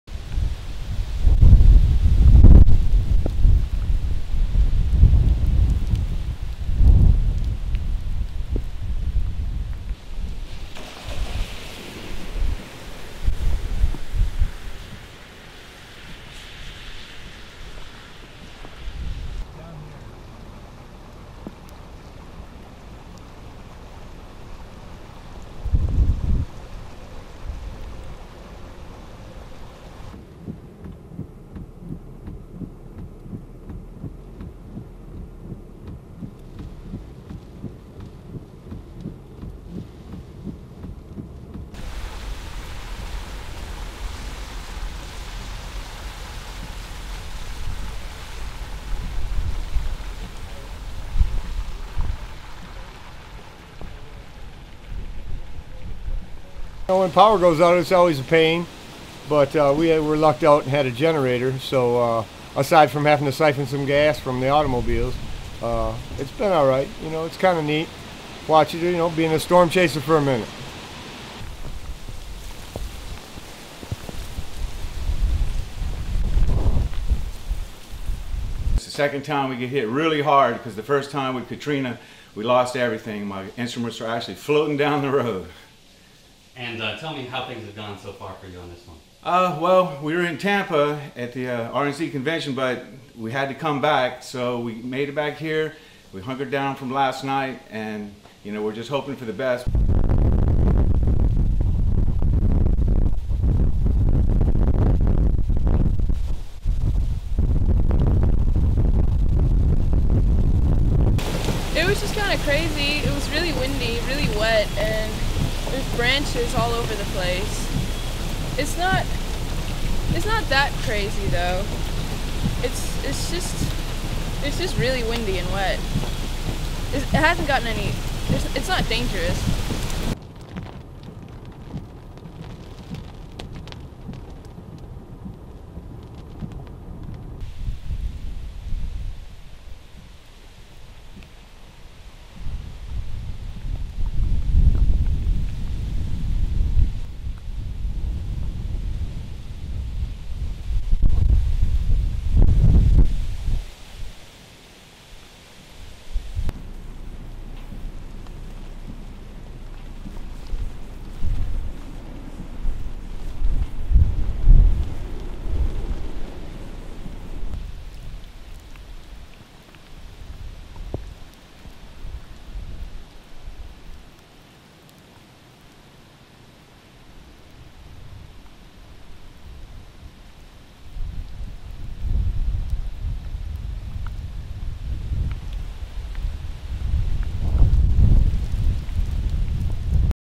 Big Easy braces as hurricane arrives seven years after Katrina
(29 Aug 2012) Hurricane Isaac lashed New Orleans on Wednesday, flooding roads, damaging homes and stranding dozens of people.
The storm came ashore around midnight Tuesday night, and continues to pound the Crescent City and environs with hurricane force winds and sheets of rain.
Storm surge driven by Hurricane Isaac overtopped a levee early on Wednesday in a thinly-populated part of Plaquemines Parish, south of New Orleans.
More than half a million people were without power across the state's southern parishes, including more than 300,000 in New Orleans and its suburbs, power provider Energy reported.
Though Isaac wasn't as strong as Katrina, evacuations were mandatory in about a half-dozen parishes.
Coastal communities were largely abandoned after evacuation orders.
Elsewhere, the storm drove sheets of rain through the nearly deserted streets of New Orleans as a population mindful of the powerful punch dealt by Hurricane Katrina seven years ago waited for the storm to get out of their lives.
Some residents, like John Cressman, were left without power and fuel supplies.
"When power goes out it's always a pain.  But, we lucked out and had a generator.  So, aside from having to syphon some gas from the automobiles, it's been all right," Cressman said.
Others had their livelihoods and businesses disrupted.
"This is the second time we get hit really hard because the first time with Katrina we lost everything, my instruments were actually floating down the road," said guitar shop owner, John Cressman.
Despite this, many were relieved the storm wasn't as strong as Hurricane Katrina.
"It's not that crazy though. It's just really windy and wet. It's not dangerous," said Vicky Hermannsdottir as she ran through the rainy streets with a friend.
Forecasters said the storm could drop up to 20 inches (50 cms) of rain, though city of New Orleans spokesman Ryan Berni said only minor street flooding and fallen trees were reported overnight.
Thousands of law enforcement officers and Louisiana National Guard troops were poised for possible rescue efforts.
Roadways in the city's eastern sector were reported flooded and trees were down.
In the city's centre section, residents remained watchful and anticipated the worst as rains and wind continued to blast the region.